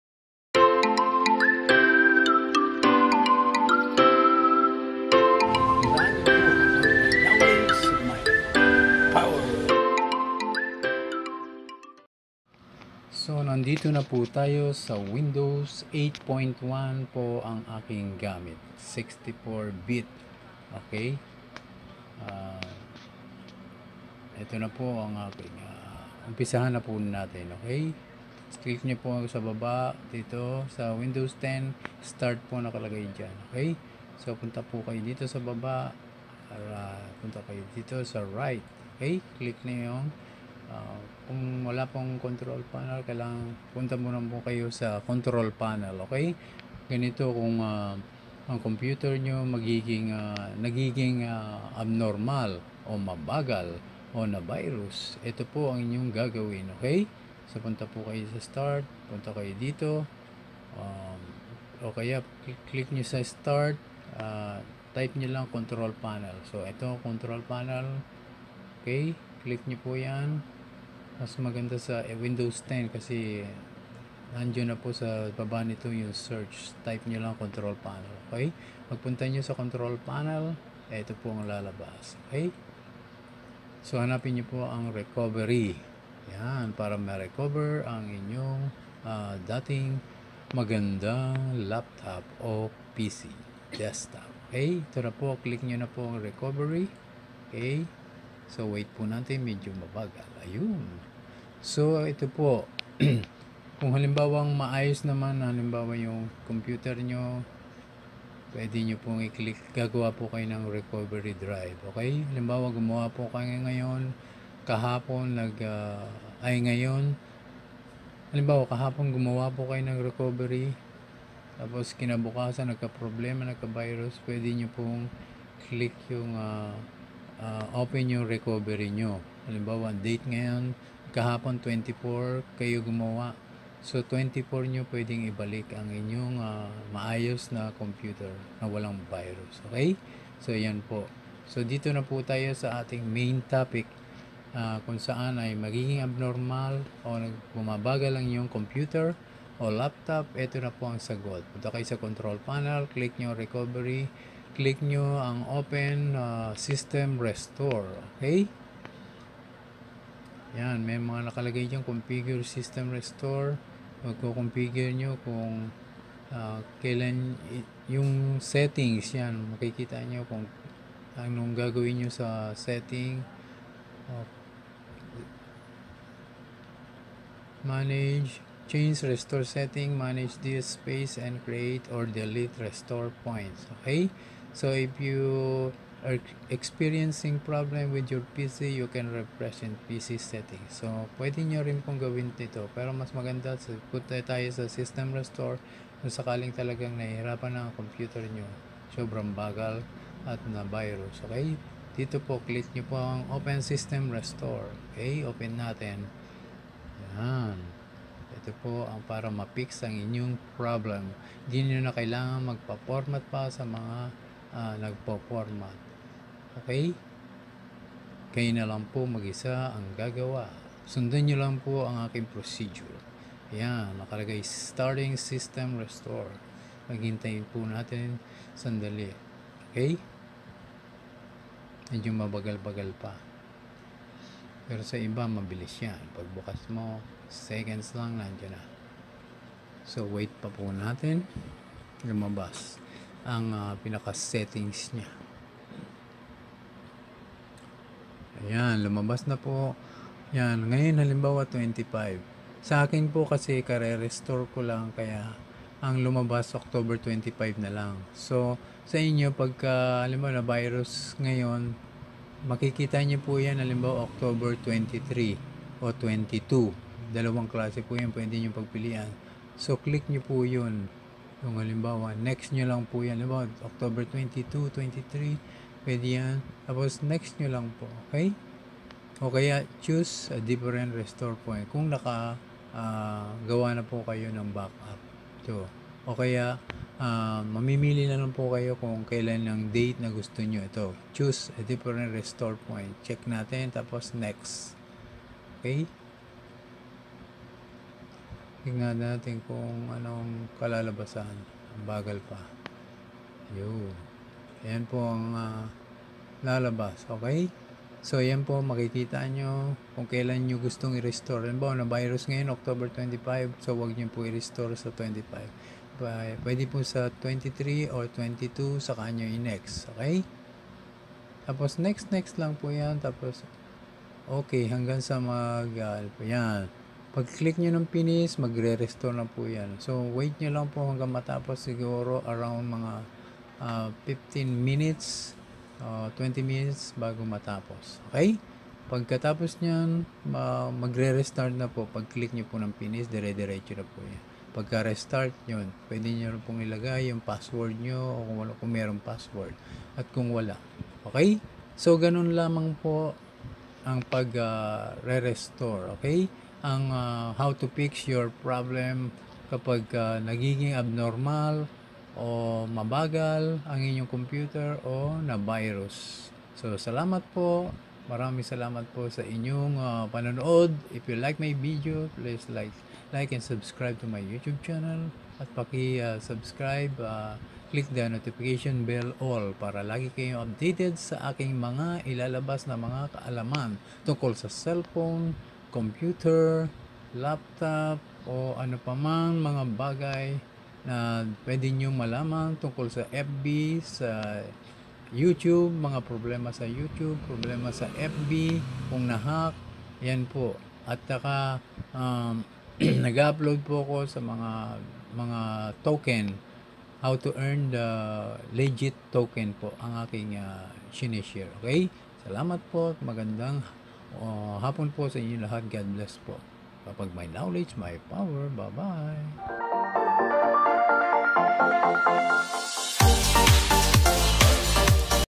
Mabagal na PC or Laptop o Na-Virus? Eto ang Solution. No need windows.
Panoorin po natin palagi ang mga problemang tulad nito. Na madalas na nagyayari sa lahat ng may Laptop or Pc. Na na-virus at mabagal na o nag-hahang. God bless po sa lahat.  Matututunan mo dito ang isang kaalaman tungkol sa Laptop or Pc.